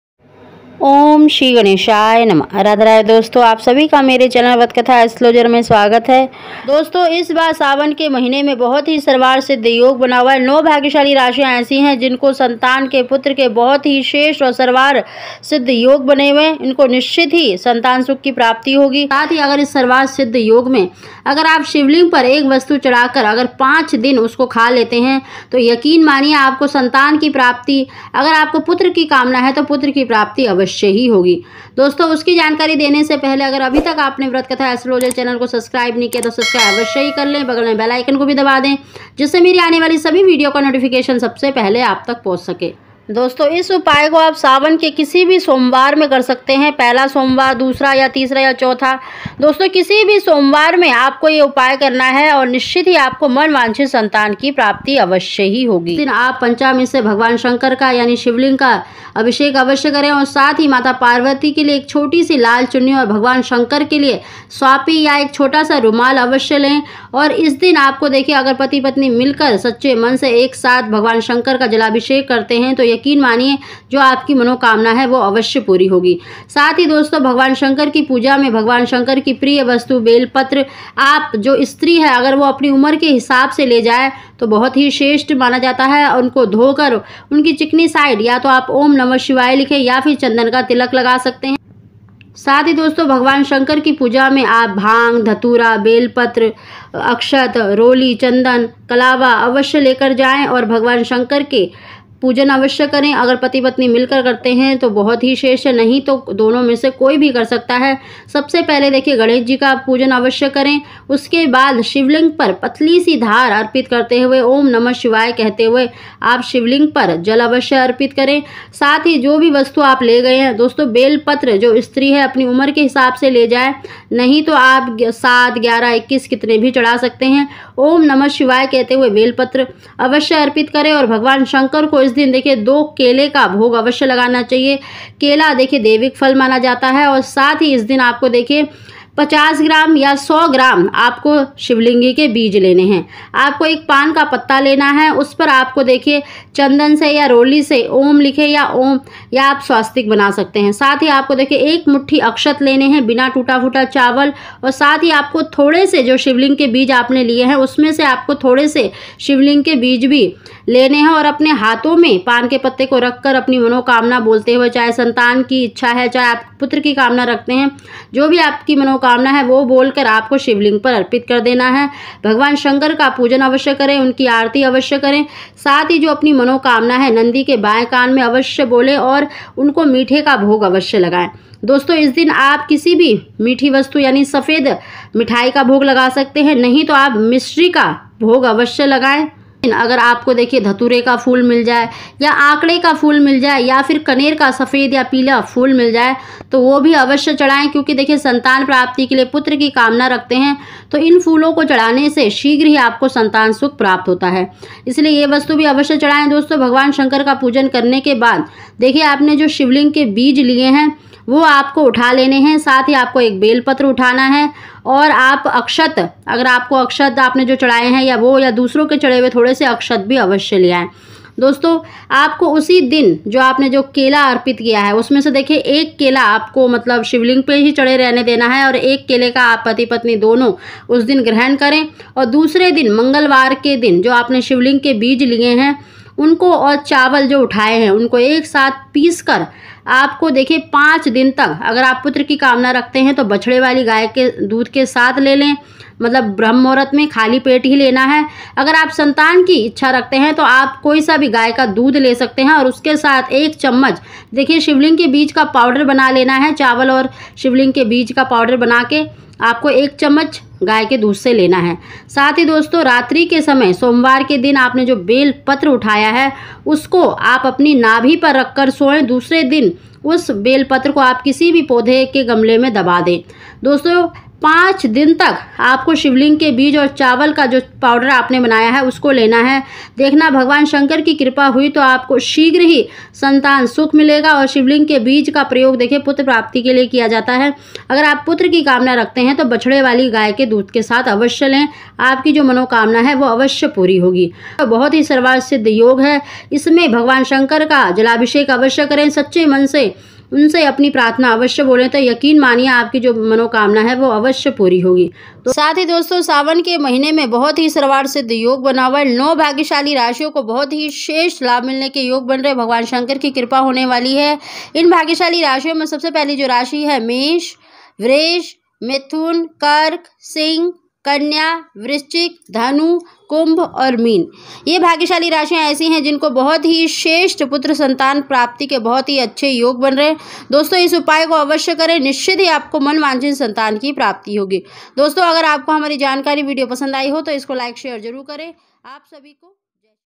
19 अगस्त सावन का आखिरी सोमवार शिवलिंग पर चढ़कर 5 दिन खा लेना 1 चीज शीघ्र होगी पुत्र संतान प्राप्ति
19 अगस्त सावन का आखिरी सोमवार शिवलिंग पर चढ़कर 5 दिन खा लेना 1 चीज शीघ्र होगी पुत्र संतान की प्राप्ति